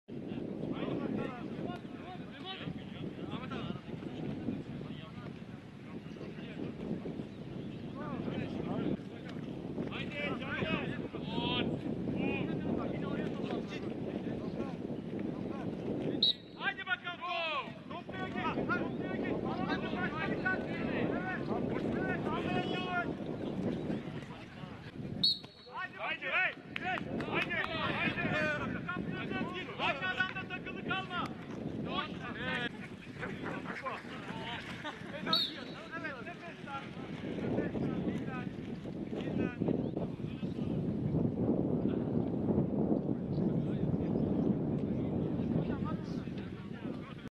Aaron Appindangoye Kocaelispor'a Katıldı! Yeşil-Siyahlılar Düzce'de Hazırlıklarını Hızlandırıyor!
Kocaelispor Haberleri- Trendyol 1. Lig ekiplerinden Kocaelispor, Düzce'de sezon hazırlıklarını akşam antrenmanıyla sürdürdü. Gabon Milli Takımı kampından dönen Aaron Appindangoye de idmanda takımla çalıştı.